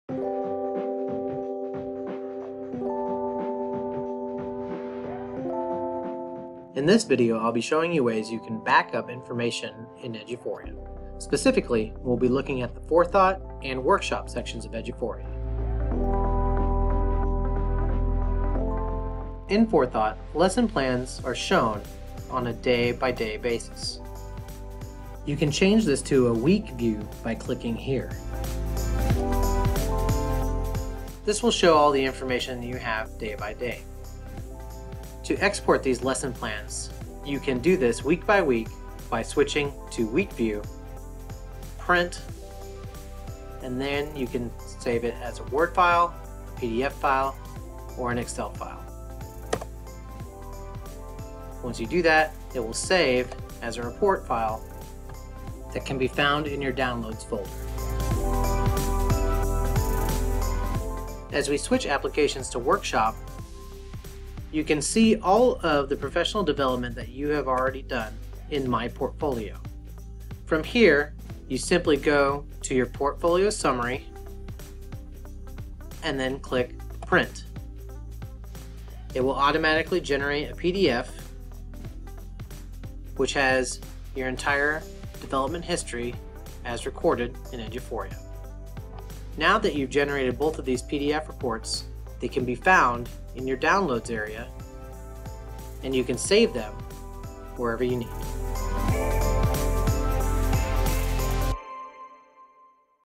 Backing Up PD and Lesson Plan Data from Eduphoria
This video will show you how to export data from Eduphoria Forethought and Eduphoria Workshop as pdf files to save for future use.